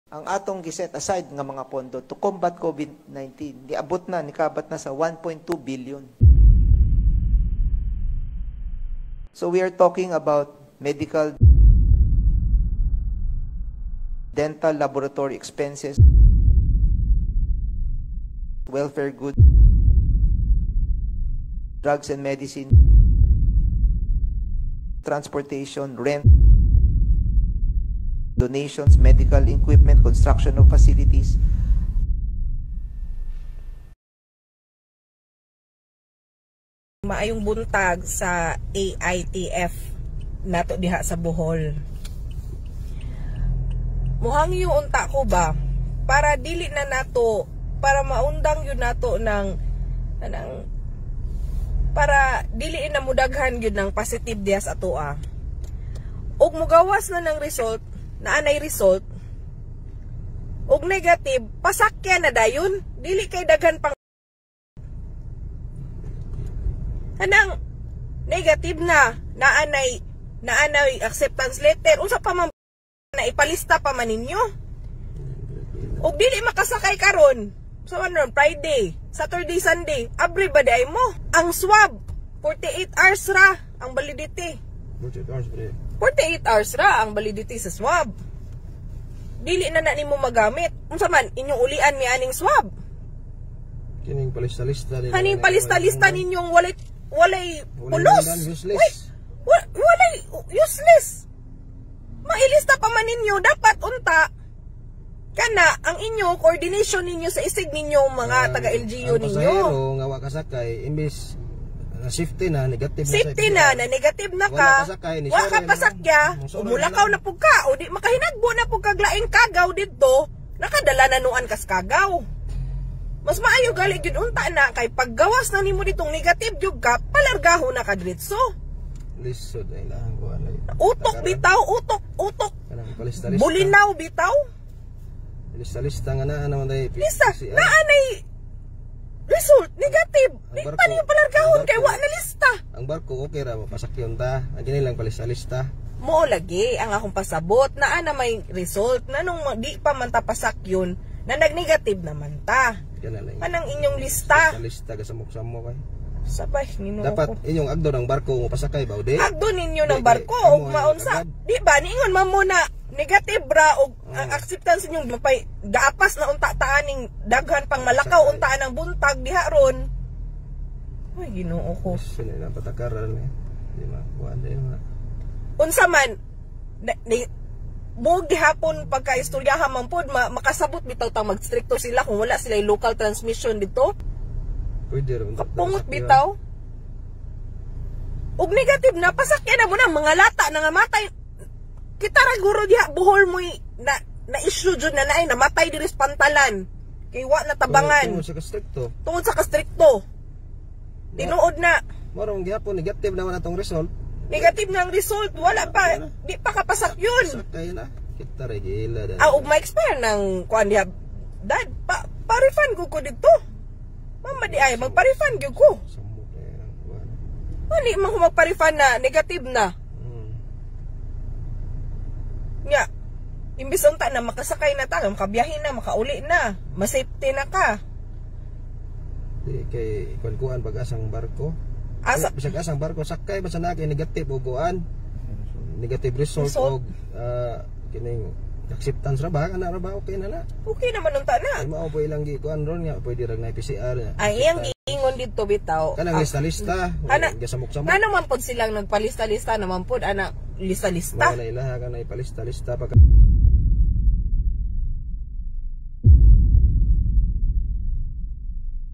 THE ART OF YAP (KASAKIT SERYE SA MGA BOL-ANON)
Isa ka babae nagpa ambit sa iyang kasagmuyo ug kasakit sa pamaagi sa Gobernador sa Bohol nga Si Art Yap. Tan-awa ang maung Video.